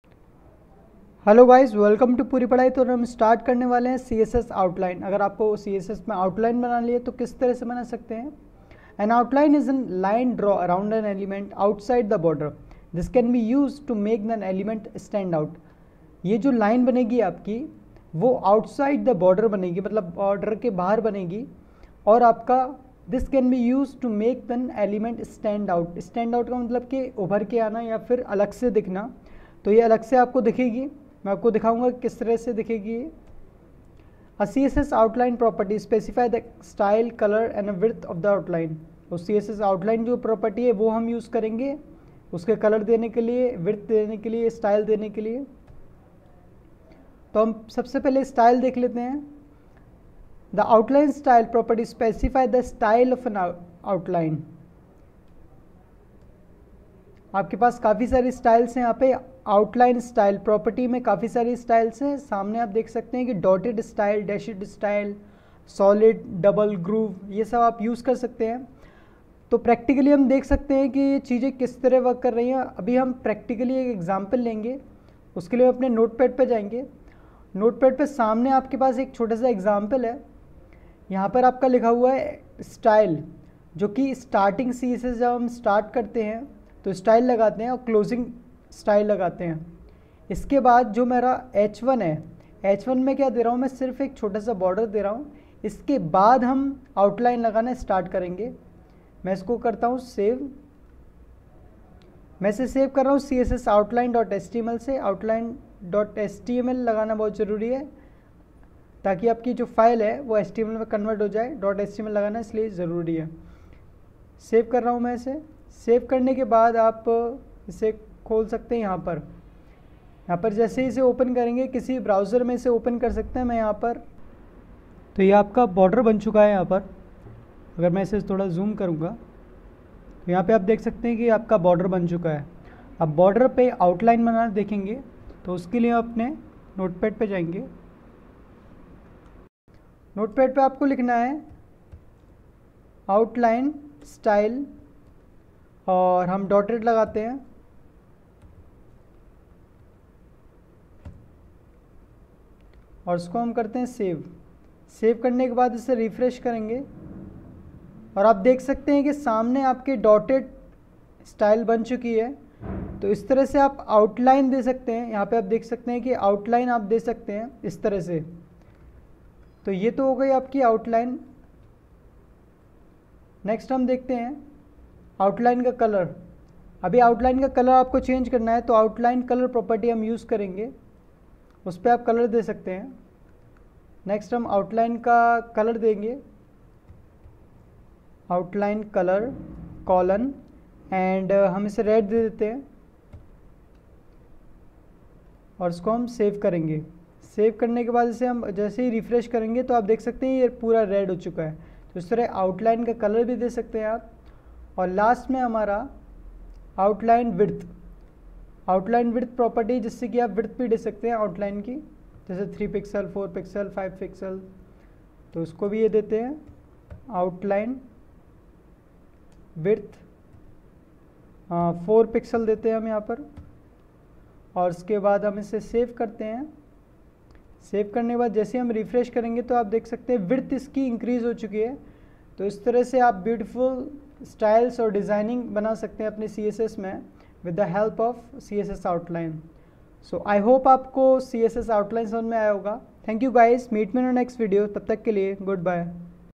CSS Outline-Hindi
What is the use of CSS Outline and how to use it to make website more beautiful.
We will also cover
The outline-style property can have one of the following values:
dotted - Defines a dotted outline
dashed - Defines a dashed outline
solid - Defines a solid outline
double - Defines a double outline
groove - Defines a 3D grooved outline. The effect depends on the outline-color value
ridge - Defines a 3D ridged outline. The effect depends on the outline-color value
inset - Defines a 3D inset outline. The effect depends on the outline-color value
outset - Defines a 3D outset outline. The effect depends on the outline-color value
none - Defines no outline
hidden - Defines a hidden outline
CSS Outline Color
CSS Outline Width
and more

You can download all the examples from 1st video of CSS Tutorial-CSS Overview